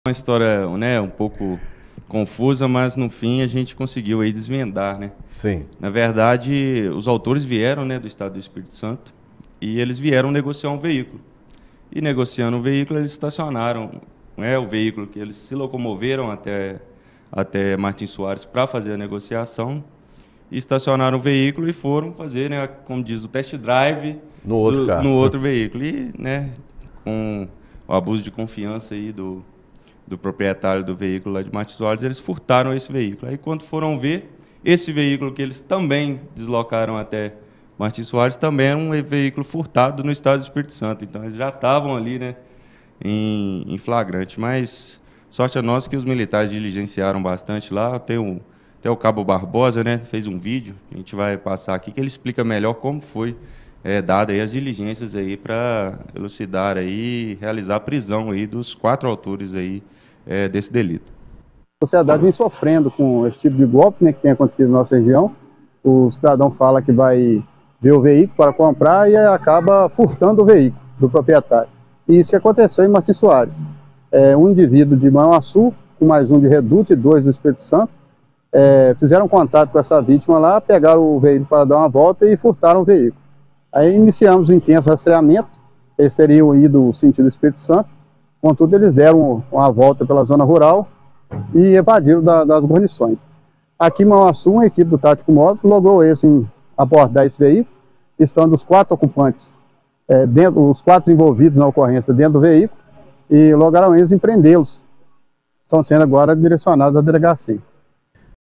Veículo furtado em Martins Soares é recuperado pela Polícia Militar
Militares recuperaram um veículo furtado em Martins Soares e prenderam quatro suspeitos na tarde desta quarta-feira (27). A ação foi realizada após uma denúncia de furto de veículo no Centro de Martins Soares. Os policiais foram ao local e conversaram com a vítima, que relatou que um homem se passou por comprador para efetuar o furto.

O autor, de 26 anos, é conhecido por práticas criminosas na região. Ele estava acompanhado de mais três comparsas. Após o furto, os suspeitos evadiram sentido Espírito Santo. A equipe policial transmitiu as informações sobre os autores e o veículo furtado via rede rádio e em todos os grupos policiais da região.

Momentos depois, os policiais militares abordaram o veículo furtado já em Manhuaçu. Os quatro suspeitos estavam embarcados e portando um simulacro de arma de fogo e uma chave mixa, possivelmente para cometer outros crimes.

Os suspeitos foram presos e encaminhados à Delegacia de Polícia Civil de Manhuaçu. O veículo furtado foi recuperado e devolvido à vítima.